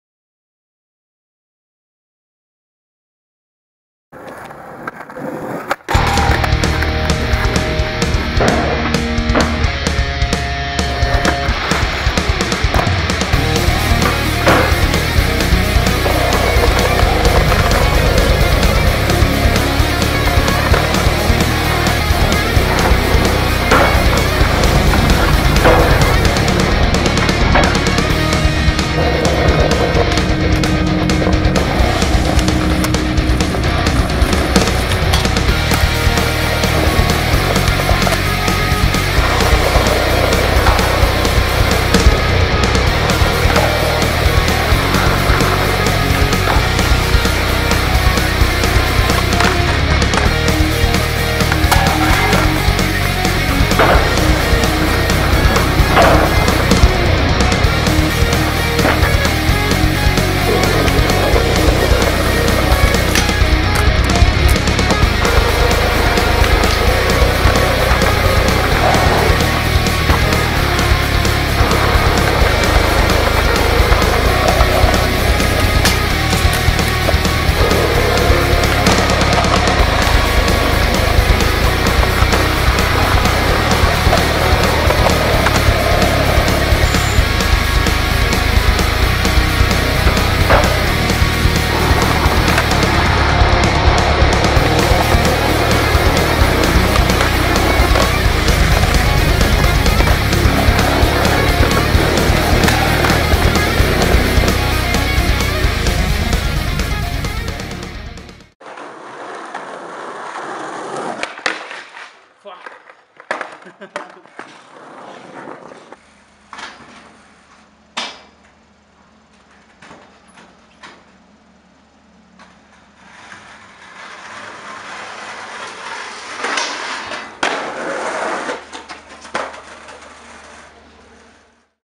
Skate Session (TESTING NEW CAMERA 🎥)
We had a Fun skateboard session testing out the New cameras.